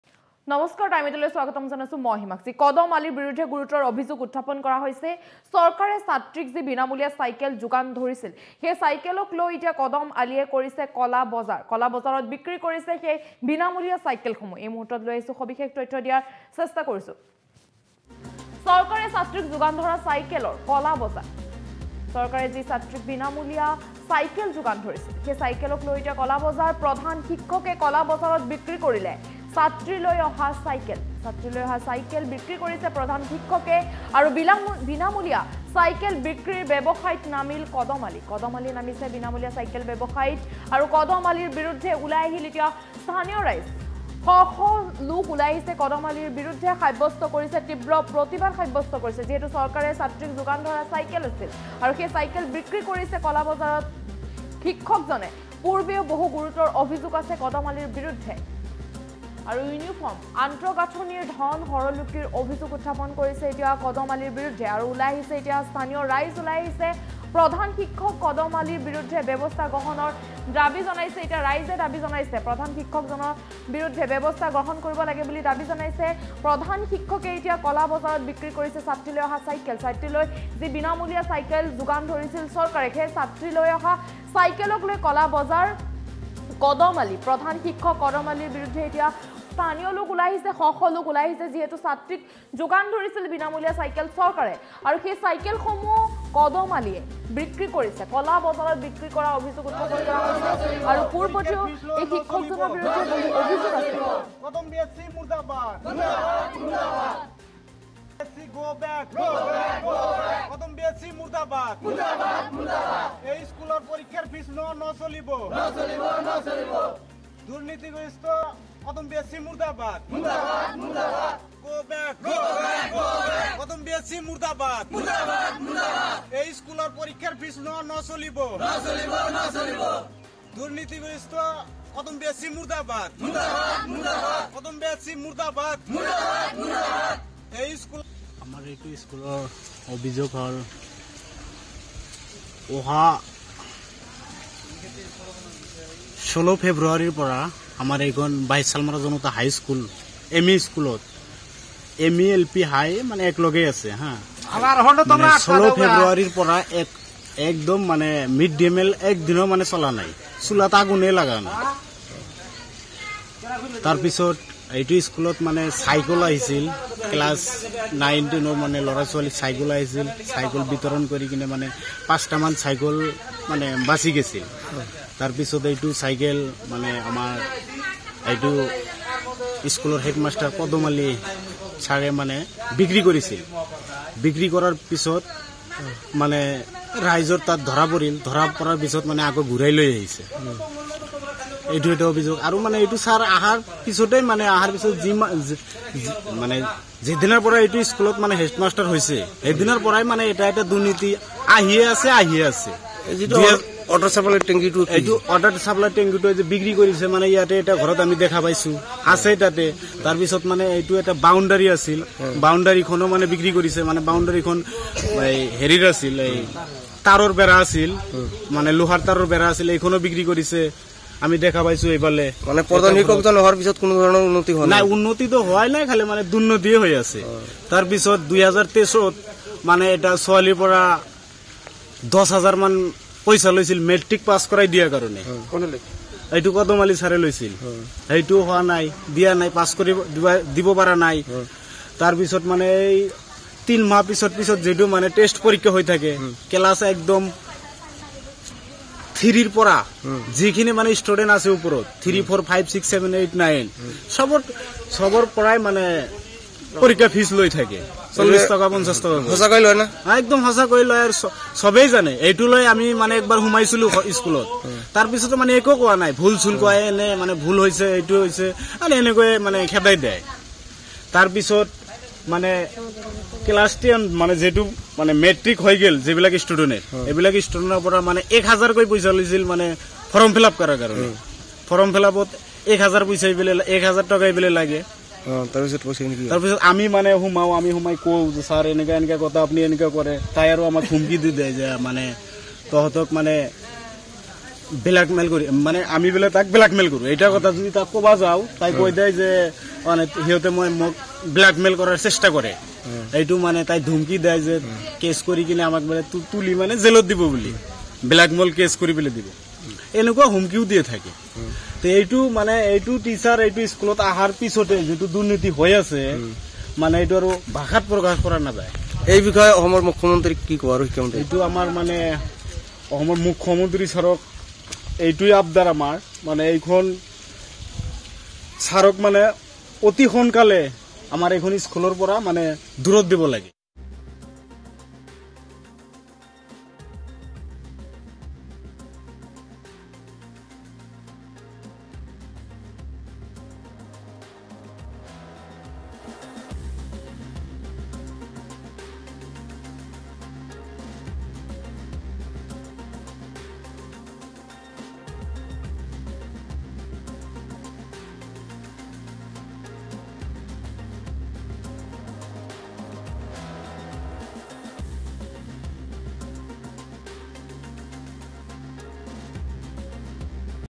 চৰকাৰে ছাত্ৰীক যোগান ধৰা চাইকেলৰ ক'লা বজাৰ
চৰকাৰে ছাত্ৰীক যোগান ধৰা চাইকেলৰ ক'লা বজাৰ । প্ৰধান শিক্ষকে ক'লা বজাৰত বিক্ৰী কৰিলে ছাত্ৰীলৈ অহা চাইকেল । বিনামূলীয়া চাইকেল বিক্ৰীৰ ব্যৱসায়ত নামিল প্ৰধান শিক্ষক কদম আলী ।